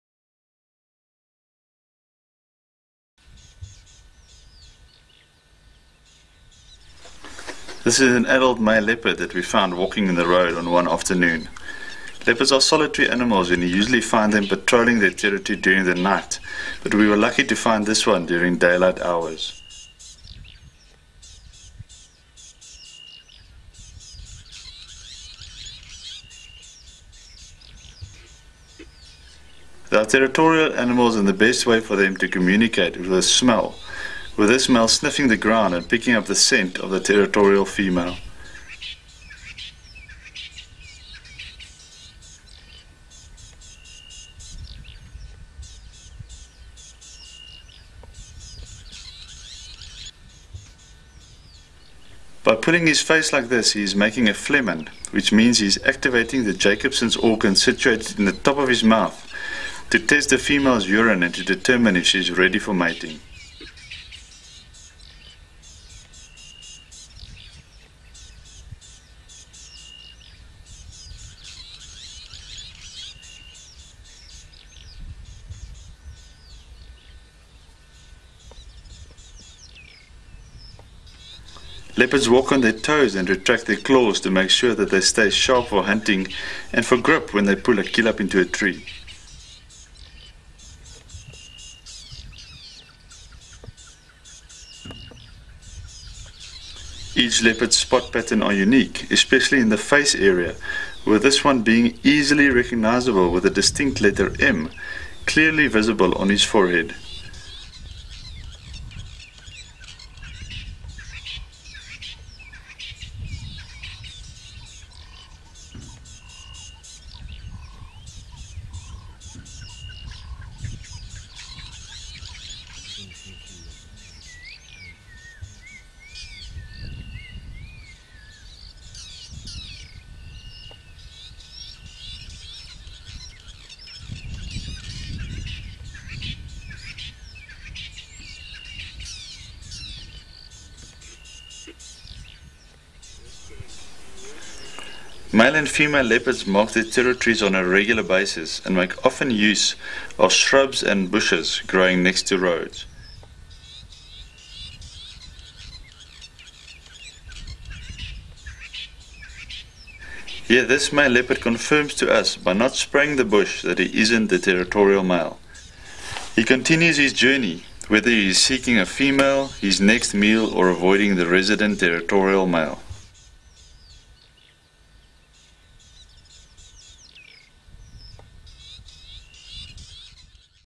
Male leopard patrolling.mp4
A male leopard (Panthera pardus) patrols a piece of South African bush in search of  a female or possibly a meal.